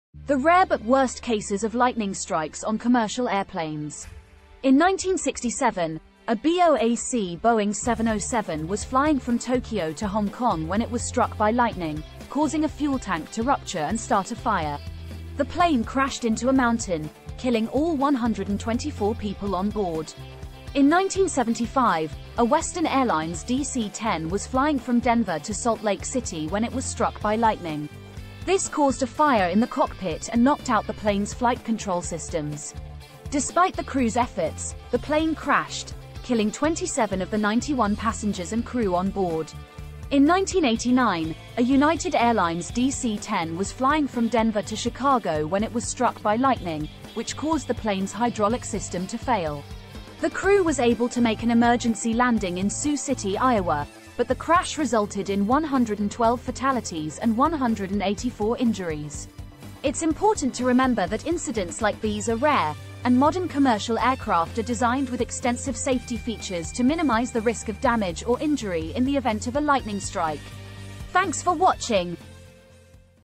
The Worst Cases of Lightning Strikes on Commercial Airplanes _ A Historical Overview
Lightning strikes on commercial airplanes are not uncommon, but thankfully, incidents resulting in significant damage or injuries are rare. However, there have been some worst-case scenarios that we can look at.
In 1967, a BOAC Boeing 707 was flying from Tokyo to Hong Kong when it was struck by lightning, causing a fuel tank to rupture and start a fire. The plane crashed into a mountain, killing all 124 people on board.
In 1975, a Western Airlines DC-10 was flying from Denver to Salt Lake City when it was struck by lightning. This caused a fire in the cockpit and knocked out the plane's flight control systems. Despite the crew's efforts, the plane crashed, killing 27 of the 91 passengers and crew on board.
In 1989, a United Airlines DC-10 was flying from Denver to Chicago when it was struck by lightning, which caused the plane's hydraulic system to fail. The crew was able to make an emergency landing in Sioux City, Iowa, but the crash resulted in 112 fatalities and 184 injuries.